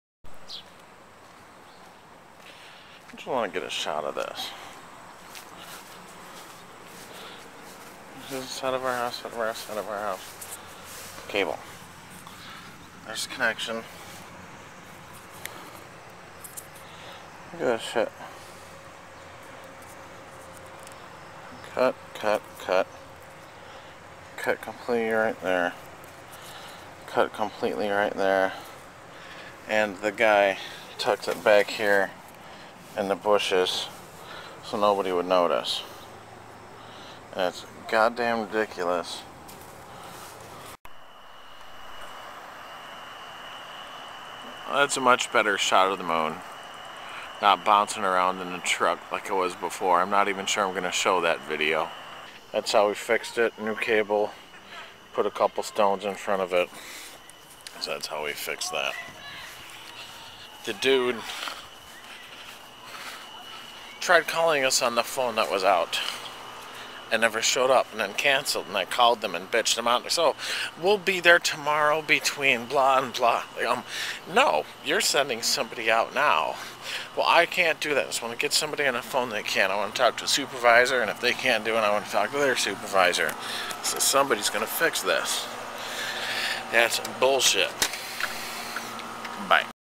cable drama
taken over the last few days . lawn guys cut our cable , then the cable guy was a dumb ass.